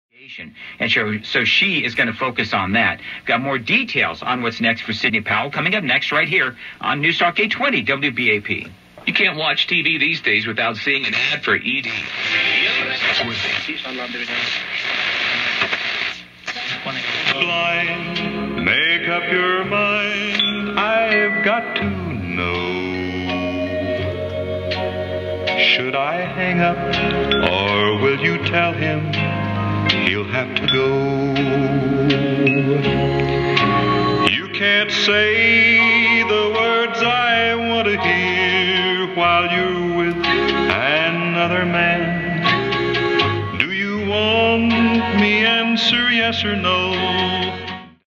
1935 Philco Model 620B
I recently refurbished this 6-tube, 3-band radio.  It took many hours to replace the electrolytic and tubular paper capacitors and out of tolerance resistors.  There were six bakelite blocks with embedded capacitors that had to be replaced.  But I was rewarded at the end with a great sounding radio.  I didn't refinish the cabinet because I like the aged look it has.  The song I tuned to is being played from Amazon Music on my computer through my AM transmitter.